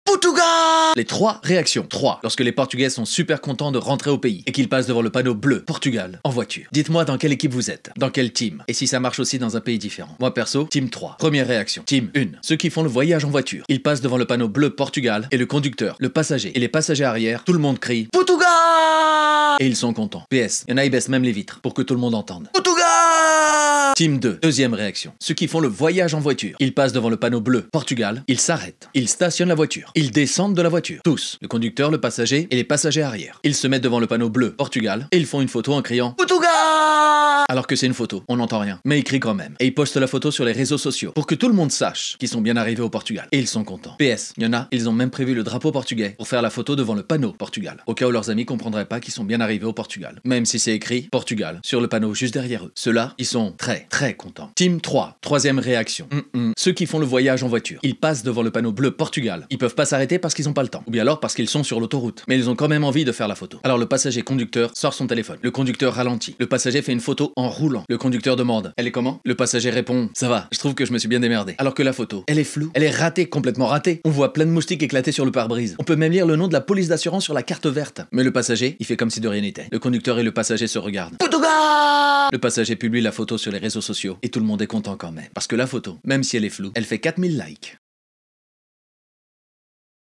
PORTUGAL, Les 3 Réactions
Portugal 🇵🇹
Les 3 Réactions principales des Portugais
lorsqu'on passe en voiture la frontière
devant le panneau bleu Portugal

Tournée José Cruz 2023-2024
nouveau spectacle :
Portugal, Voyage au Centre du Monde
Infos et résas :